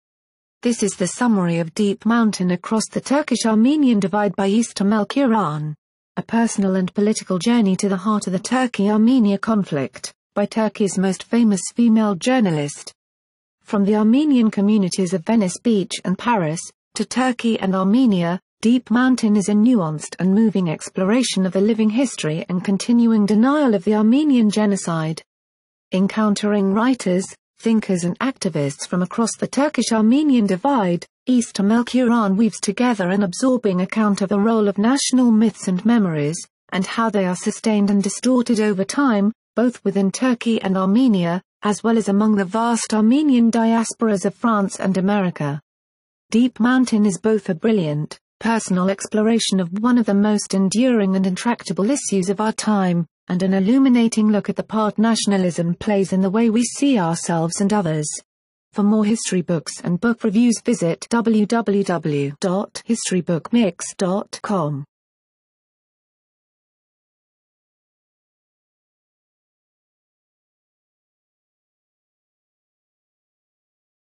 History Book Review: Deep Mountain Across the Turkish-Armenian Divide by Ece Temelkuran
This is the summary of Deep Mountain Across the Turkish-Armenian Divide by Ece Temelkuran.